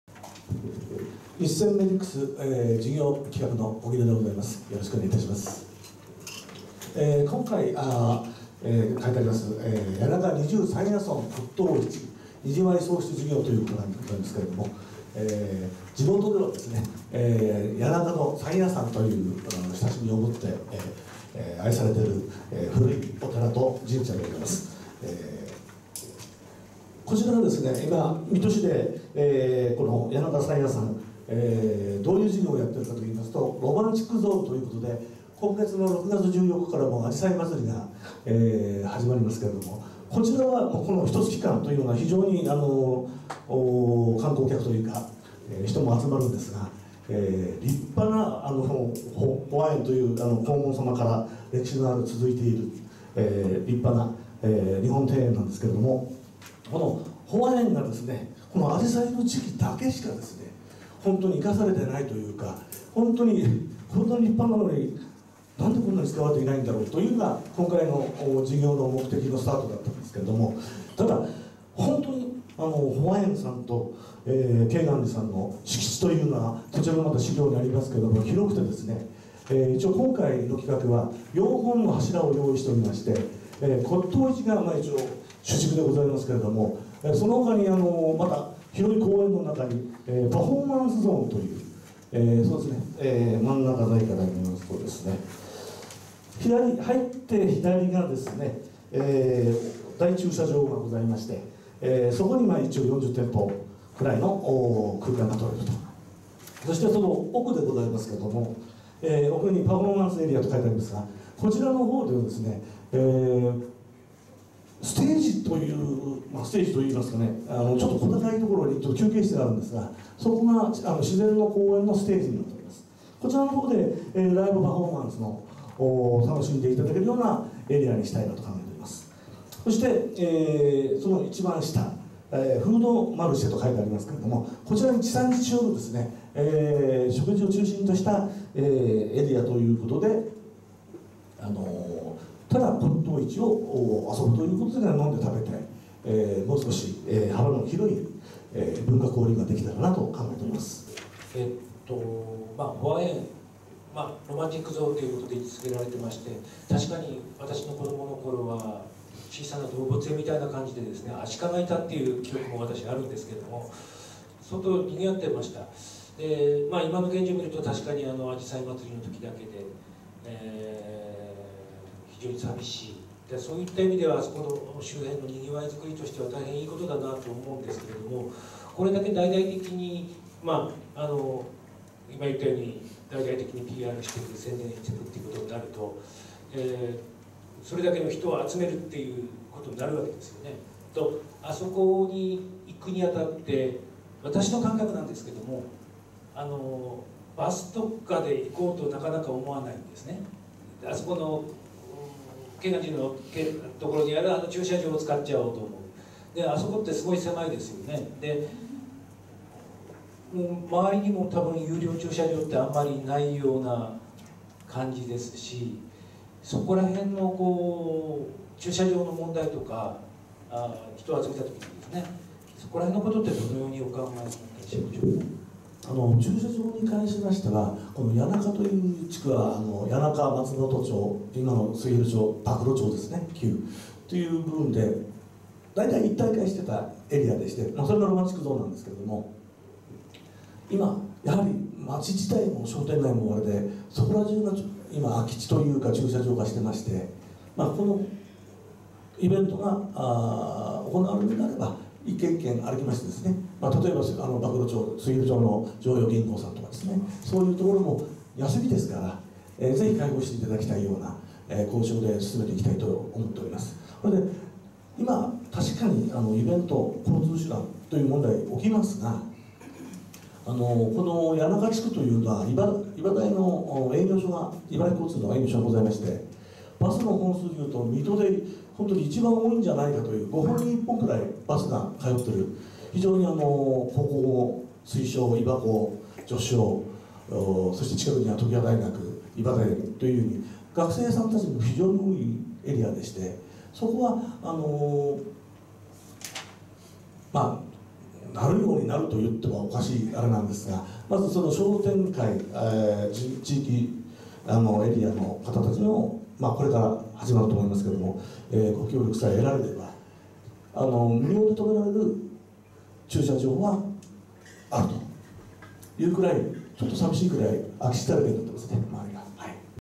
③谷中二十三夜尊骨董市賑わい創出事業（平成27年度「わくわくプロジェクト」事業プレゼンテーション）
平成27年度の水戸市協働事業提案制度「わくわくプロジェクト」の事業プレゼンテーションです。
【みと協働大作戦！「わくわくプロジェクト」】